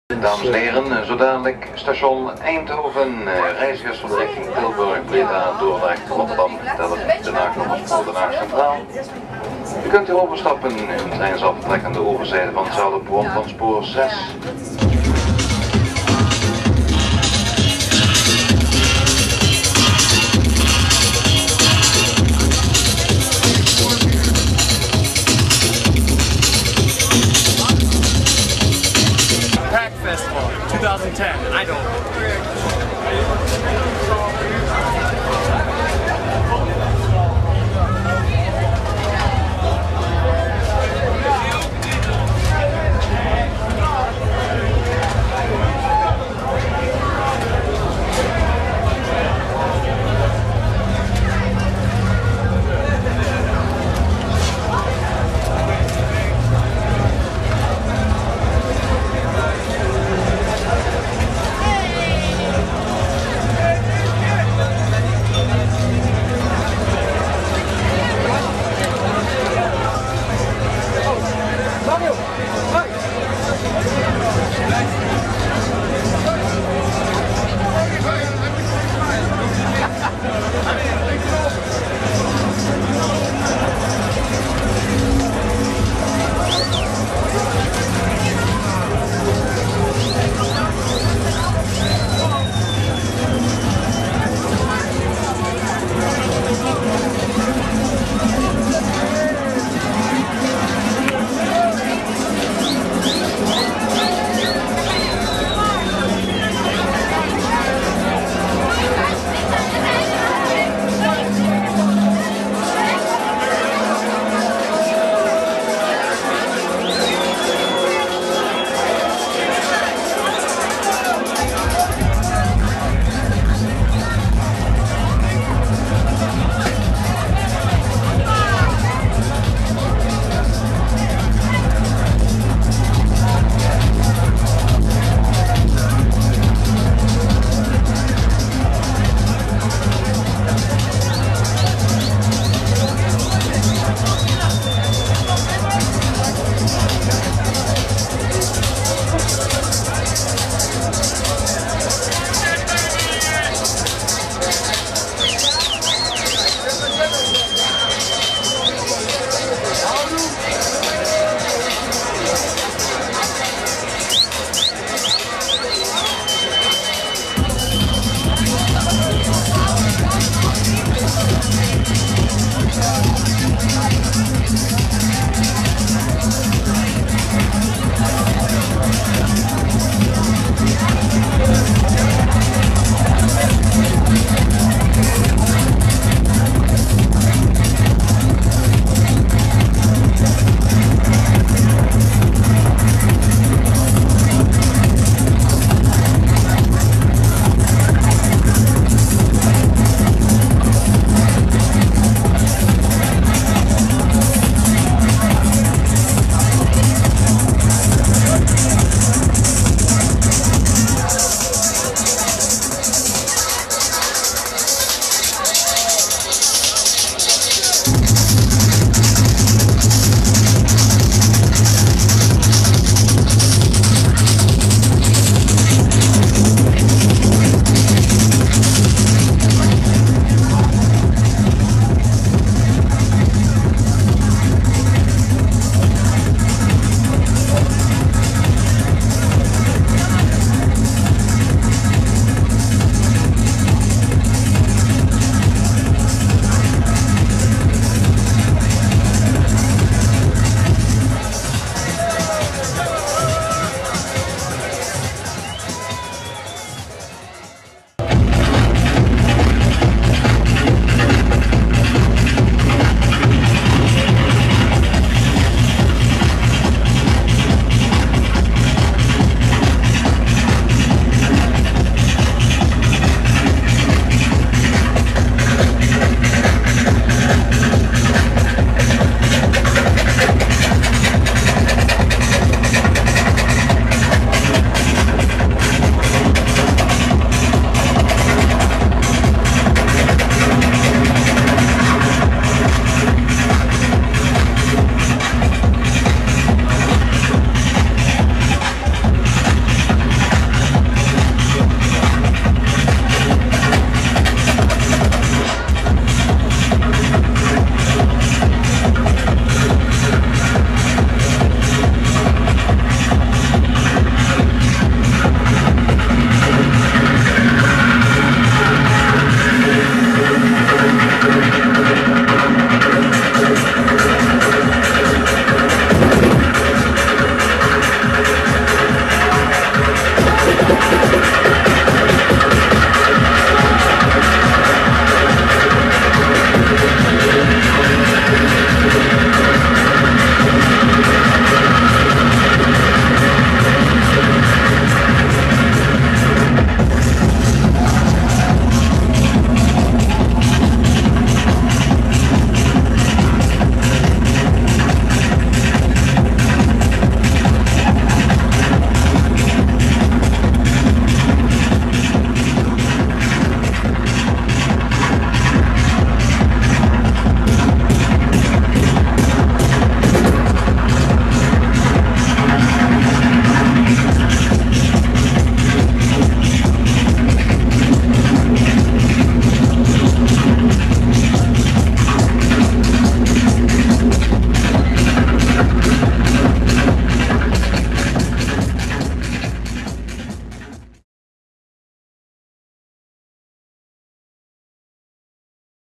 Detroit Techno Militia Solidarity Tour 2010: VLOG 4
The Detroit Techno Militia Solidarity Tour took place in April of 2010. This is the Fourth installment of the video blog that tracks DJ Seoul & T.Linder.  Here is the final performance of the tour at the PACT Festival in Eindhoven, Holland on Queensday.